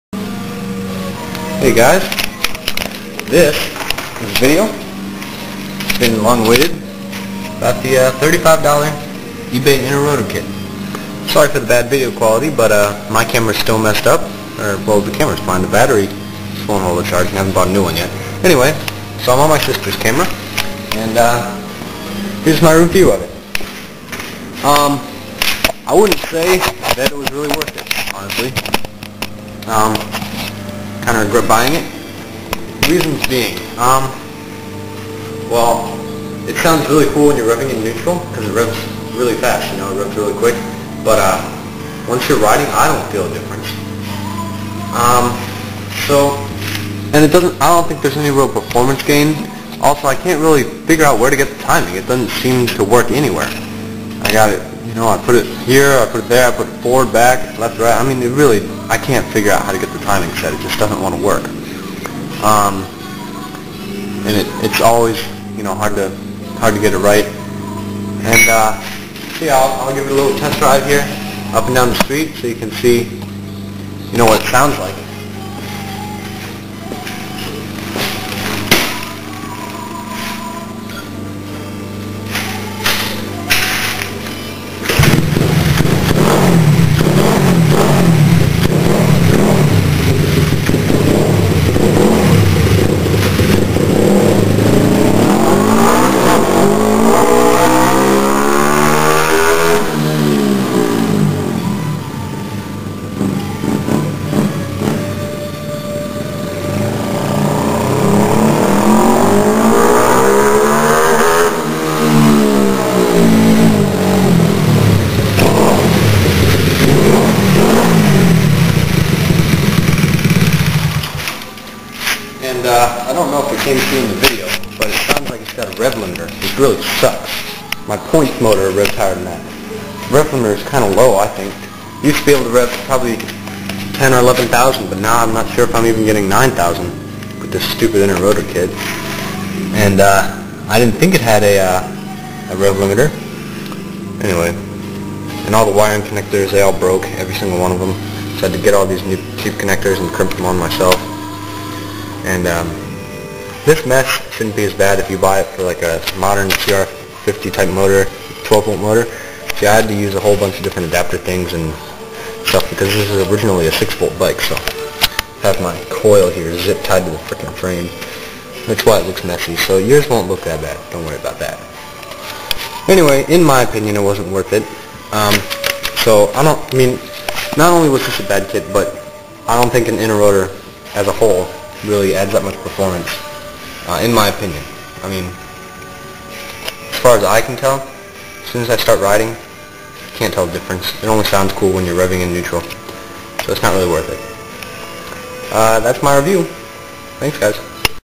eBay $35 Inner Rotor Kit Review
The eBay special is pretty cheap. The connectors were all junk, and it seems to limit the revs too low. Overall quality was low.

This is generic to all IRK's: I found that it didn't seem to actually add any performance. Once riding, I could not notice a difference. The  only noticeable change is when revving in neutral.

Use of an IRK is rider preference. In my case, I don't like the IRK. Another rider may prefer it. In my opinion, I'm sure an IRK would work fine for some riders but the eBay special should not be a first choice.